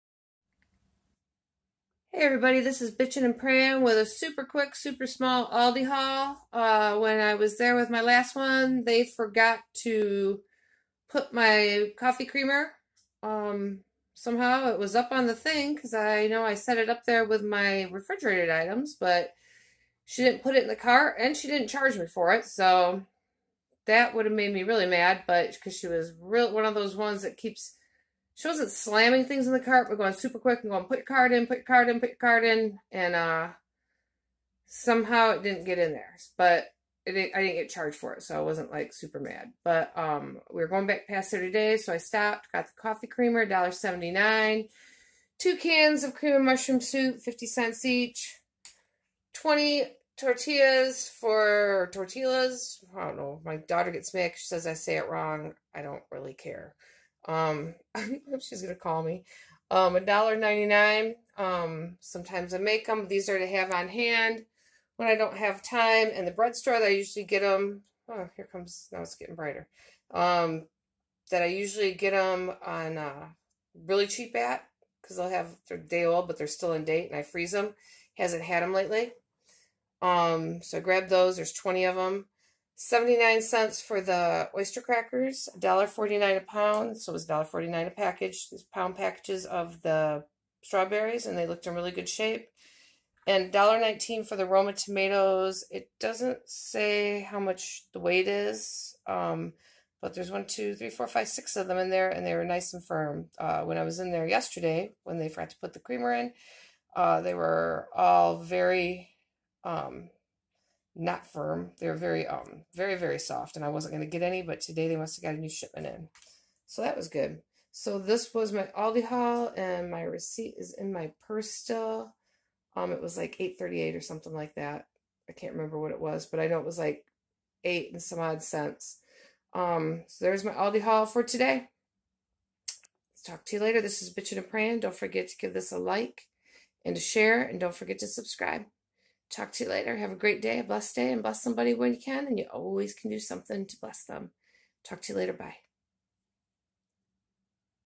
Back to Aldi's!! Mini Haul.
luckily I was going right back by there. Being Frugal and saving money on a shoestring budget, shopping at Aldi's is just one of the many things that helps me do this.